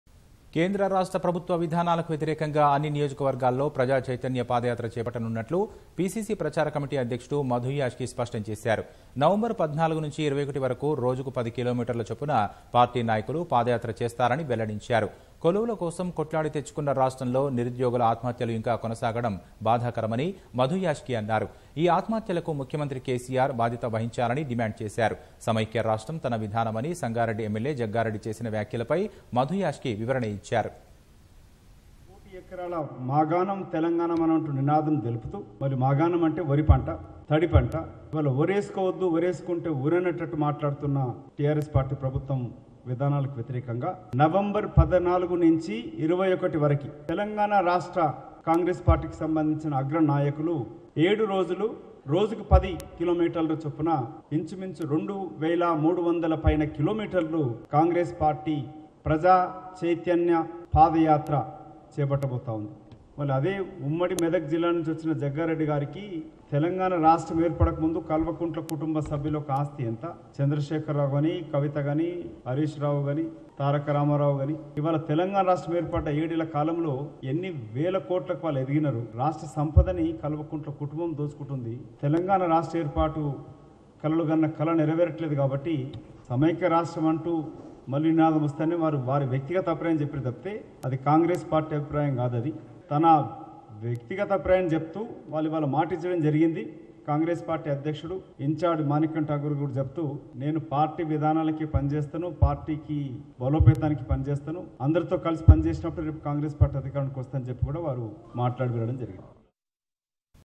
''ప్రభుత్వ విధానాలకు వ్యతిరేకంగా ప్రజా చైతన్య పాదయాత్ర'' | Madhu Yashki On Praja Chaithanya Yatra
కేంద్ర, రాష్ట్ర ప్రభుత్వ విధానాలకు వ్యతిరేకంగా అన్ని నియోజకవర్గాల్లో ప్రజా చైతన్య పాదయాత్ర చేపట్టనున్నట్లు పీసీసీ ప్రచార కమిటీ అధ్యక్షుడు మధు యాష్కీ స్పష్టం చేశారు. నవంబర్  14 నుంచి 21 వరకు రోజుకు 10 కిలోమీటర్ల చొప్పున పార్టీ నాయకులు పాదయాత్ర చేస్తారని వెల్లడించారు. కొలువుల కోసం కొట్లాడి తెచ్చుకున్న రాష్ట్రంలో నిరుద్యోగుల ఆత్మహత్యలు ఇంకా కొనసాగటం బాధకరమని మధు యాష్కీ అన్నారు. ఈ ఆత్మహత్యలకు ముఖ్యమంత్రి కేసీఆర్  బాధ్యత వహించాలని డిమాండ్  చేశారు. సమైఖ్య రాష్ట్రం తన విధానమని సంగారెడ్డి ఎమ్మెల్యే జగ్గారెడ్డి చేసిన వ్యాఖ్యలపై మధు యాష్కీ వివరణ ఇచ్చారు.